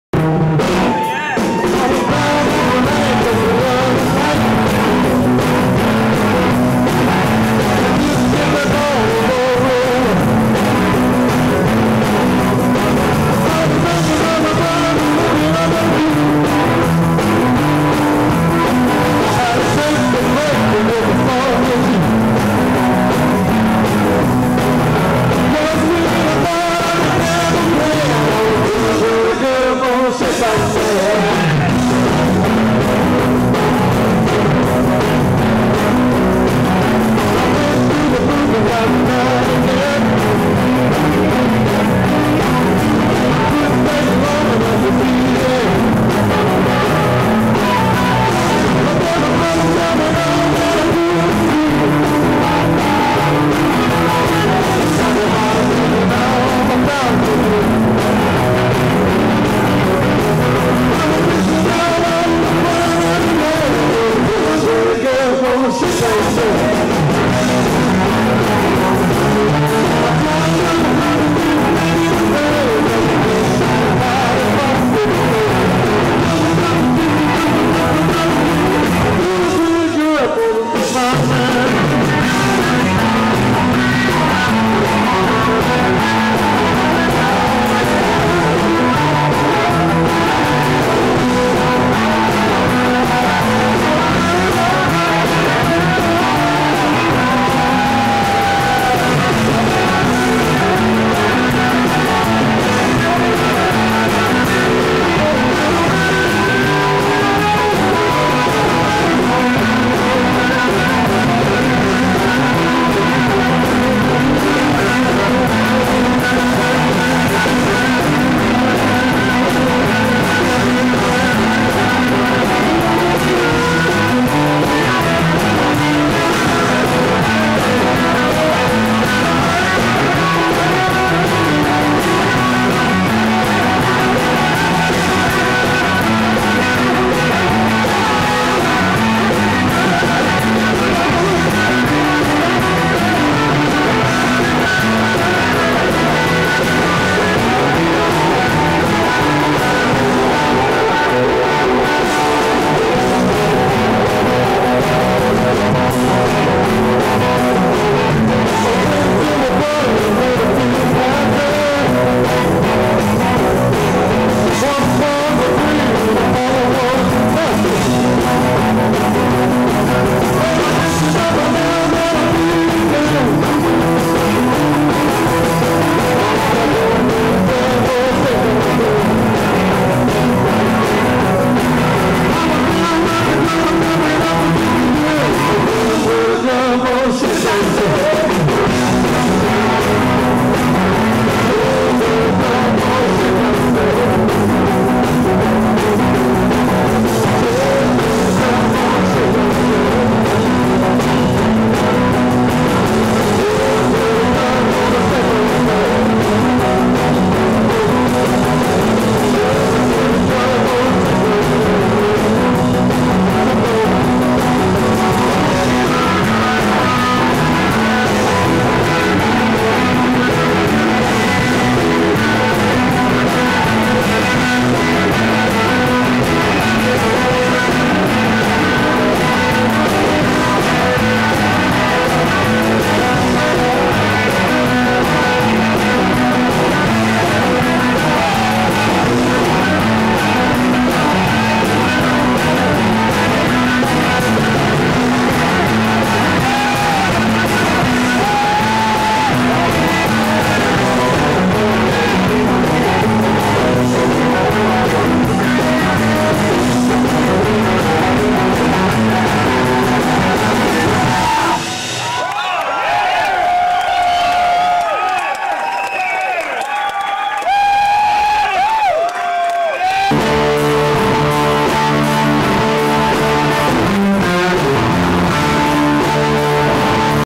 Lil Doyle Bramhall/Arc Angels
Lil Doyle with Arc Angels from 1991 , bad sound but better than nothing